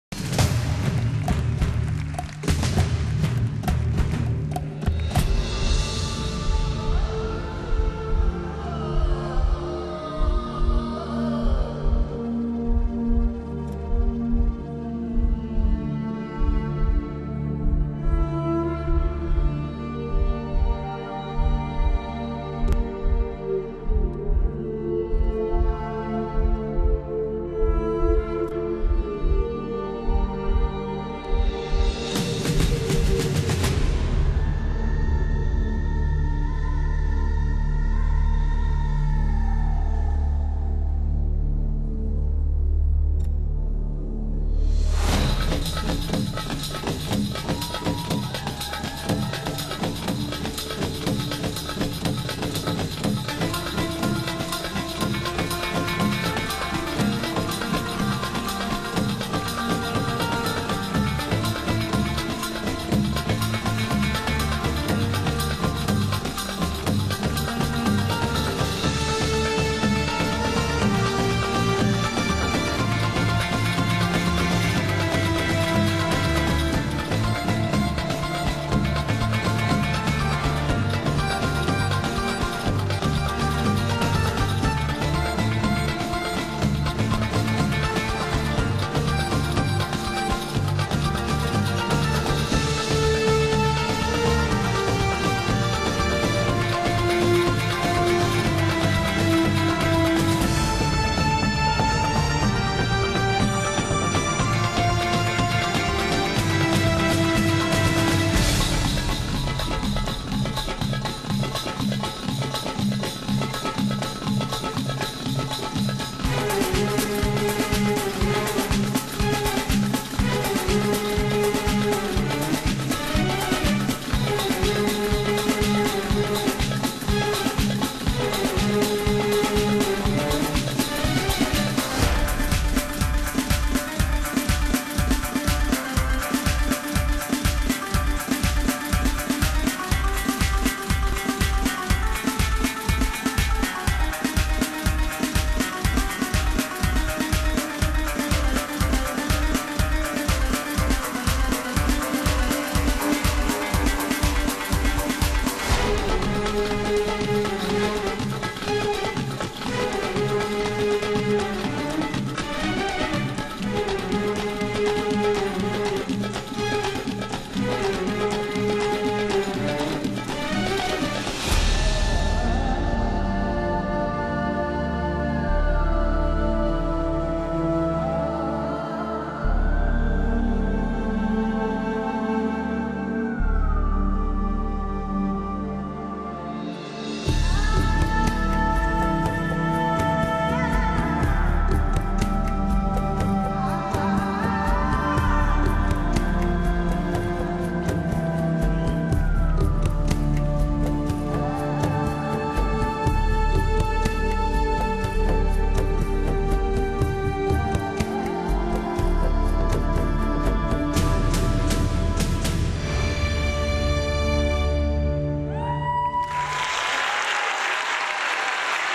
Magdans Isisvingar
Magdans med isisvingar från Malmö Dansakademis avslutningsshow 2011.